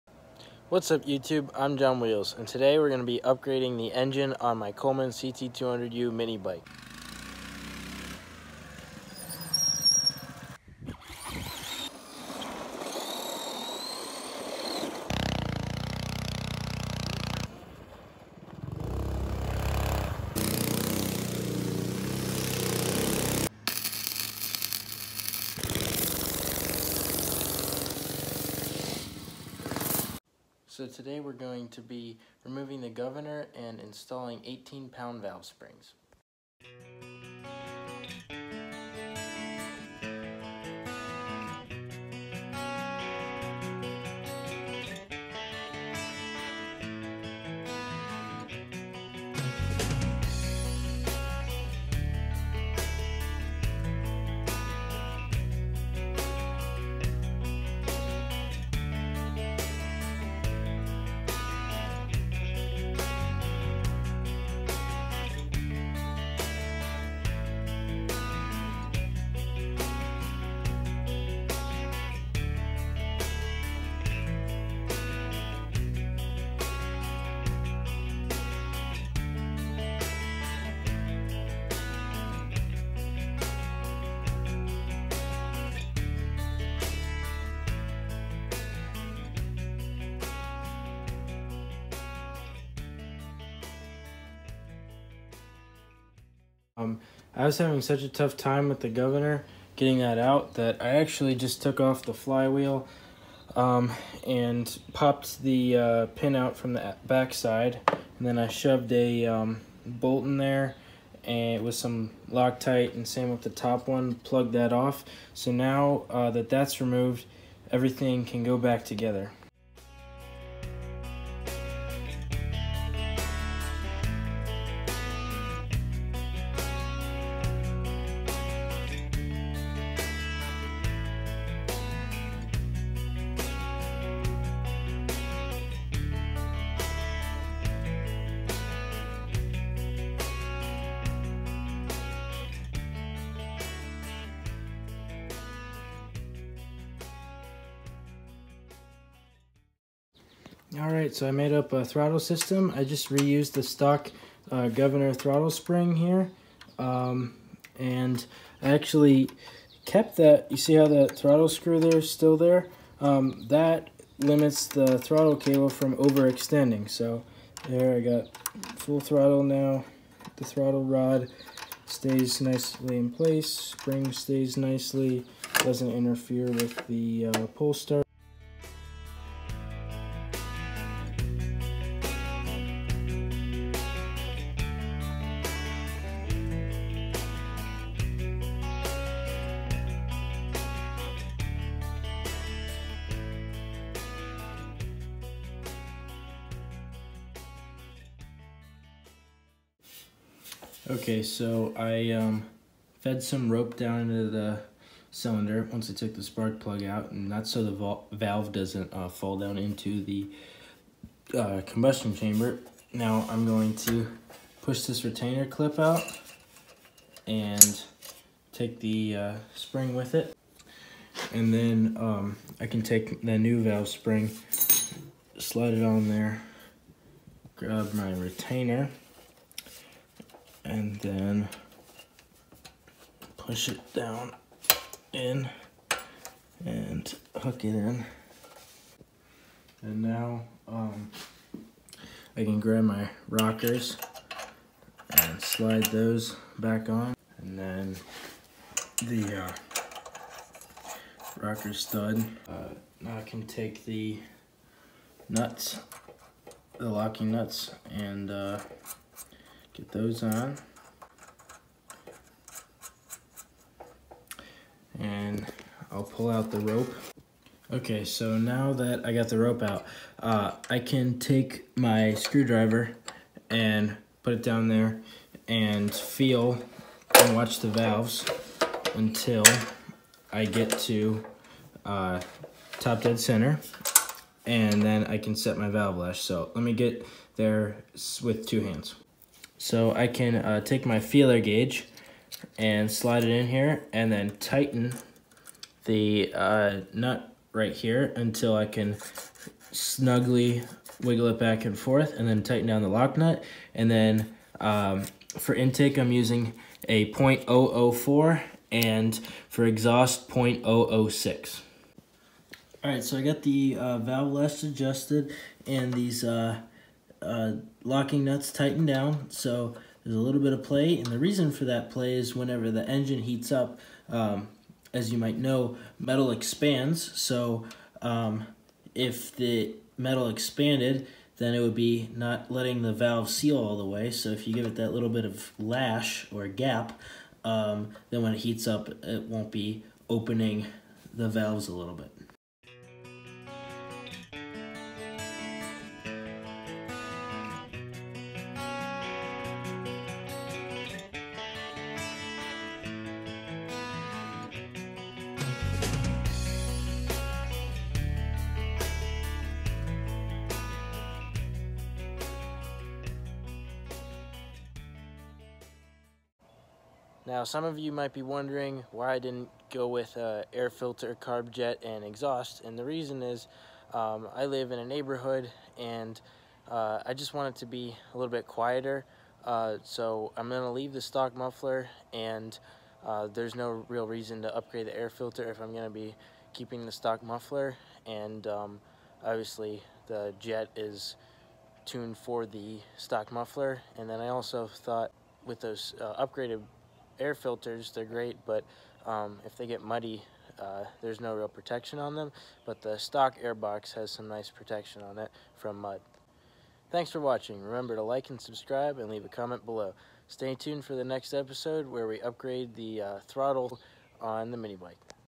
Coleman CT200u Engine Upgrades
In today's video, I remove the governor, install 18lb valve springs, and make my own throttle linkage.